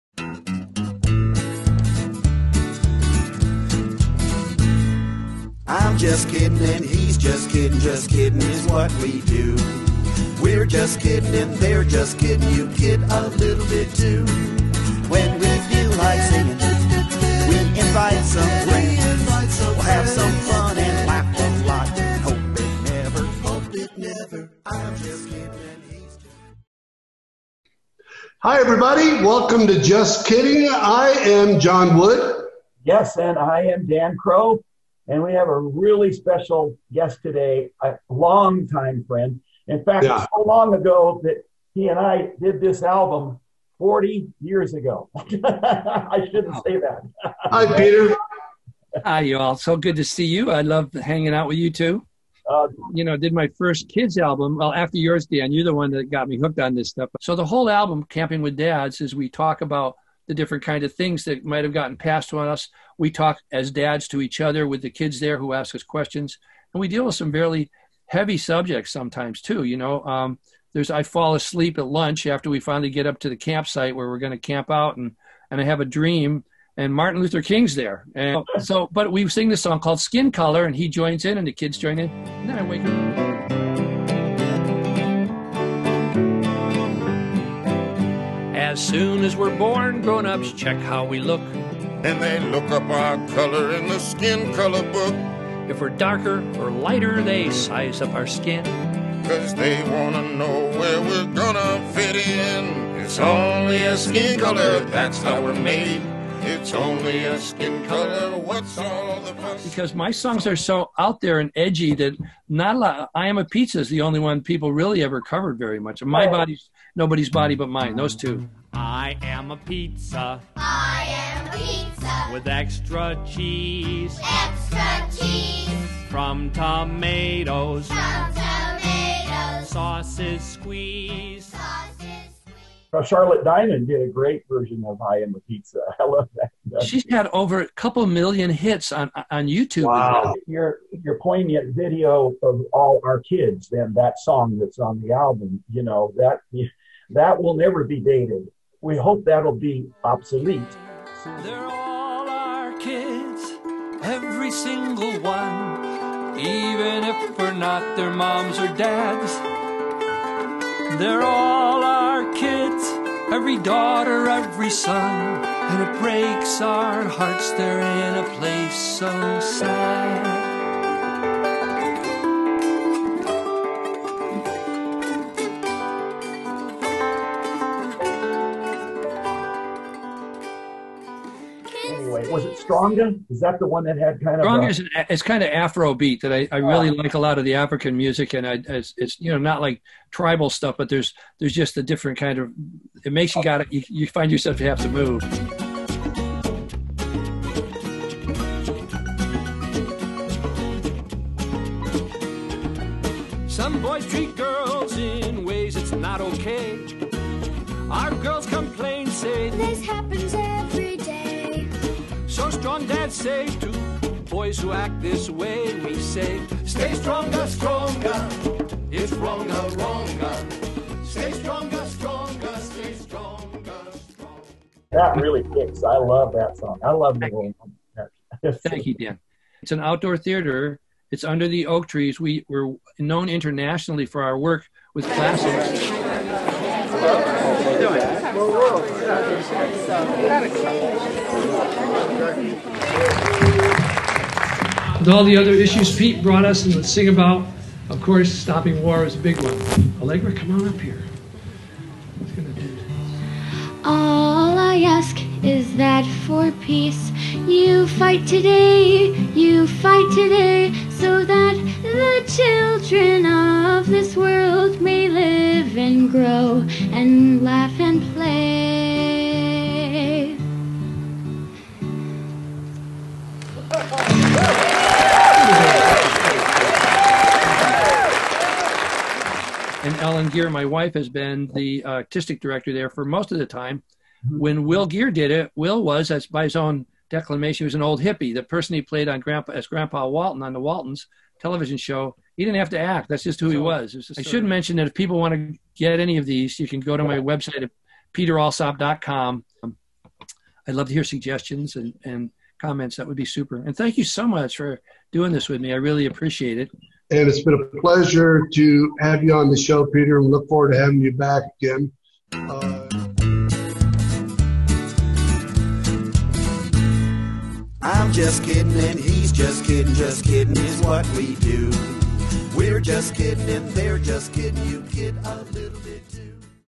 Peter Alsop Teaser
On our podcast, JUST KIDDIN', John and I talk with with our fellow singer/songwriter and long-time friend the legendary, Peter Alsop. Let this teaser lead you to our interview with him.